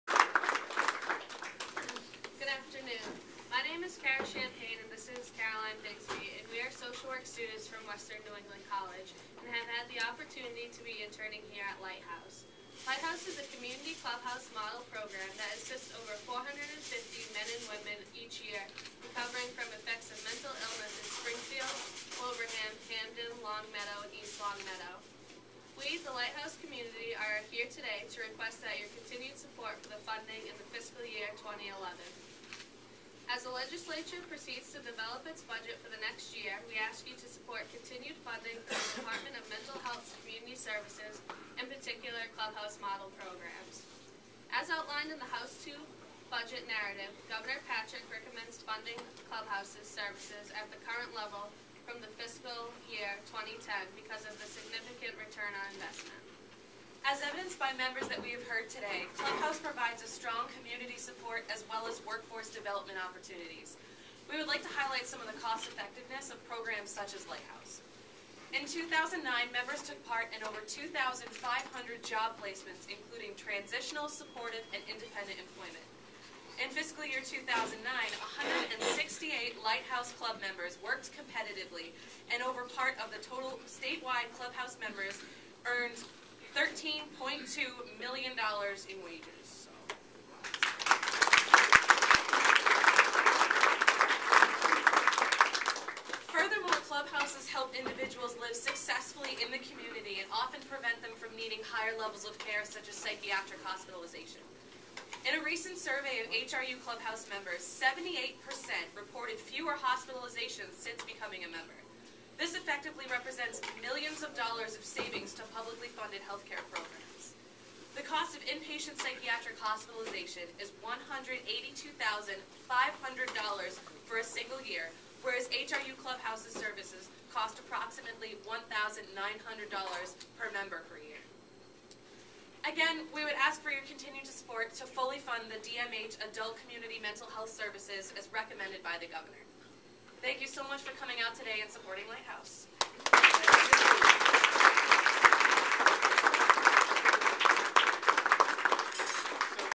Social Work Students Lobby Legislators
Western New England College Social Work students Caroline Bixby and Kara Champagne speak during a legislative reception at the Lighthouse Clubhouse in Springfield on Friday, March 26. The students are interning at Lighthouse, which provides recovery, housing, and employment support to more than 450 people with mental illness. Bixby and Champagne lobbied for continued support of programs such as Lighthouse while state lawmakers prepare to hammer out a budget for the new fiscal year.